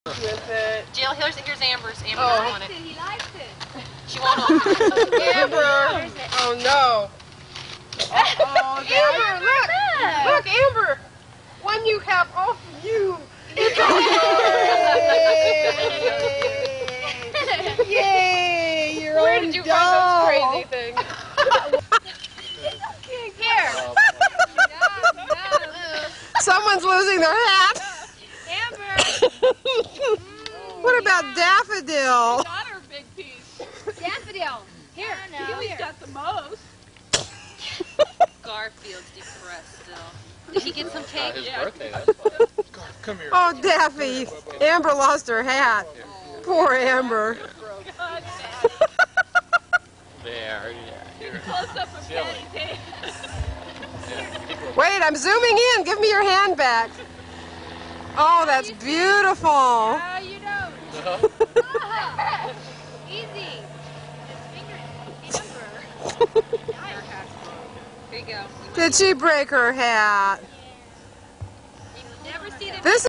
Daffi & Amber's 8th birthday party
When litter sisters, Ch. Sunnyside Daffodil and Homewood Amber had their 8th birthday, I decided it was time to celebrate!  I hope everyone enjoys our silliness as much as we and the dogs did!